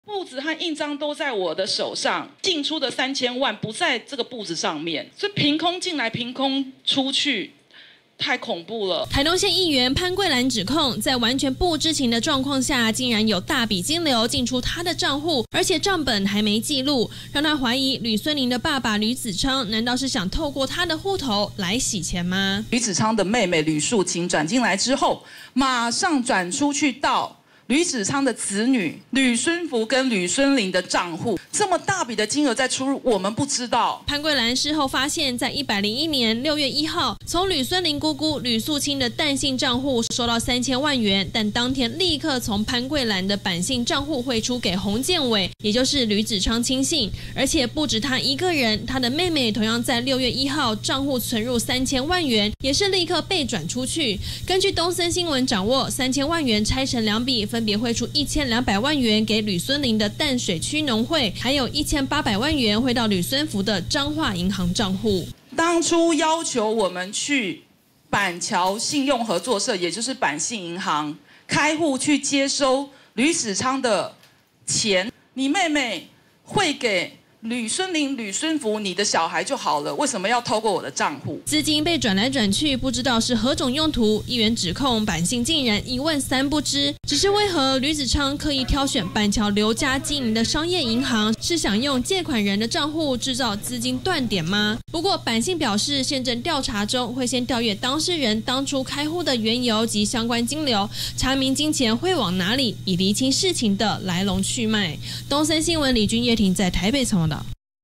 藍控呂孫綾父洗錢！ 受害議員3千萬「憑空」遭匯入 匯出@newsebc
台東縣議員潘貴蘭今天(26號)召開記者會，指控國策顧問、民進黨前立委呂孫綾的父親呂子昌，涉嫌透過她的板信商業銀行帳戶，進行洗錢，因為她發現，在完全不知情的狀況下，竟然在同一天，有3000萬元的金流，被匯入以及匯出。

11/10~11/24 敬請鎖定51頻道！